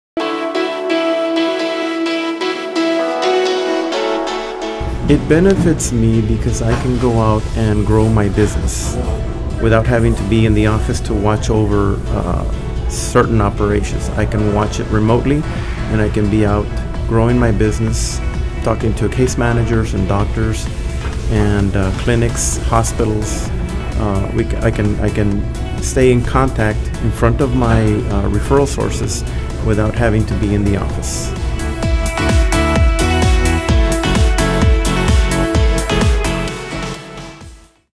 How Axxess Home Health Agency Software Helps Grow Your Business
Find out from Presidente Homecare CEO Romeo Valladares how Axxess keeps you compliant with best billing practices to make sure you stay compliant and get paid on time.